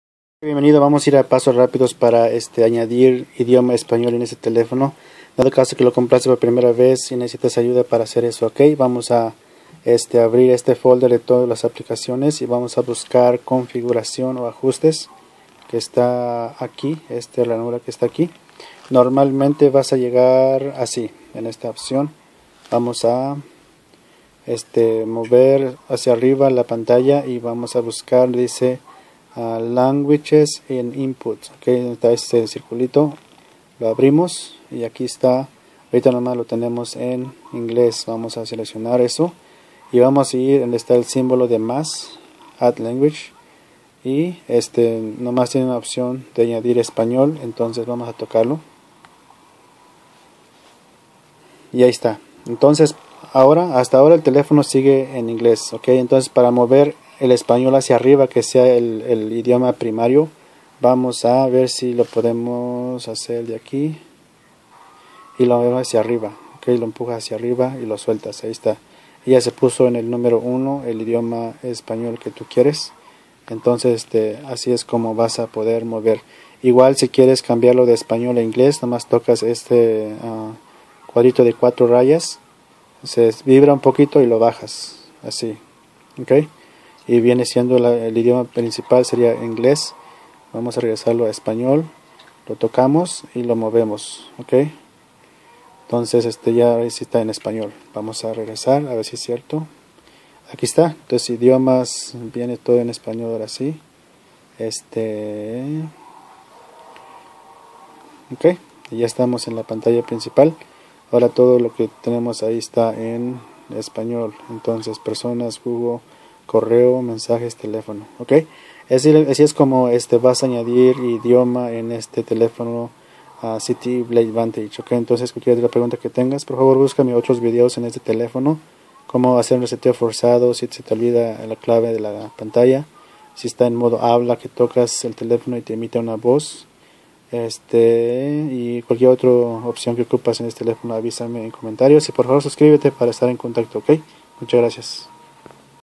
Cambiar idioma Español a ZTE blade vantage Modelo Z839 Verizon
Pasos rapidos de como cambiar el idioma a este teléfono ZTE blade vantage de Verizon prepaid modelo Z839